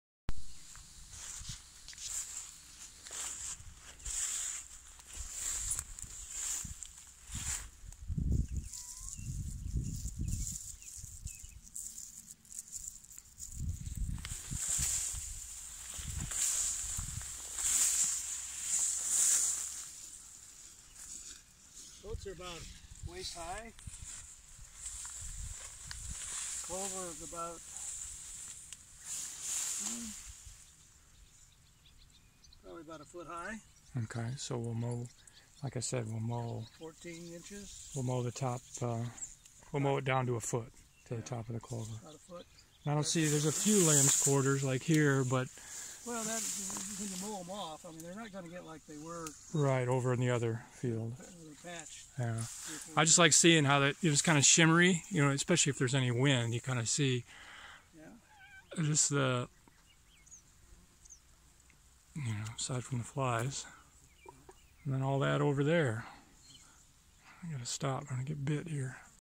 Standing at the Edge of a Field of Oats and Clover
There's about 90 acres in total in their first year of transition to certified organic production. Oats and red clover were drilled together, with the oats acting as a "nurse crop". Now that they've done their job of helping to establish a relatively weed free stand of clover, I'll mow them and leave the residue in place to act as a "green manure". Next year the clover will return, fixing additional nitrogen for the corn that I'll plant in the spring of 2021.